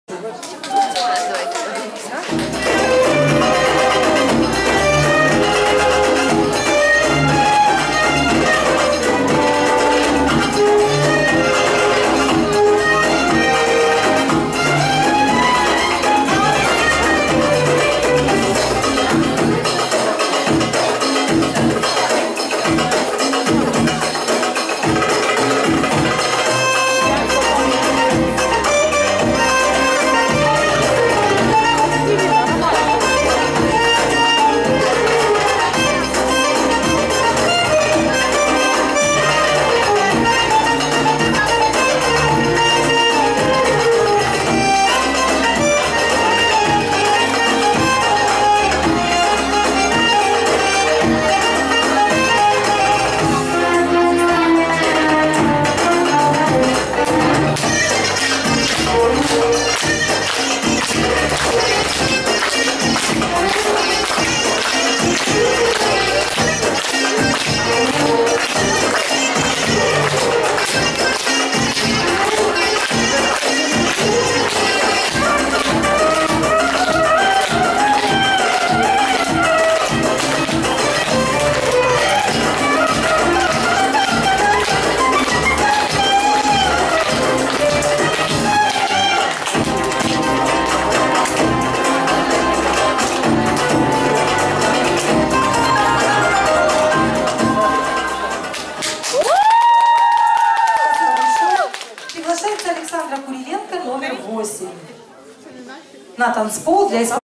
Кузнецова Александра, Мистерия звезд 2015, СВТ "Amjad" (Амджад), руководитель Ольга Ахапкина
Кузнецова Александра, беби классика 1 место

Мистерия звезд 2015
Открытый кубок Харькова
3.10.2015
Студия восточного танца "Amjad" (Амджад)
г. Полтава
руководитель, хореограф, постановщик - Ольга Ахапкина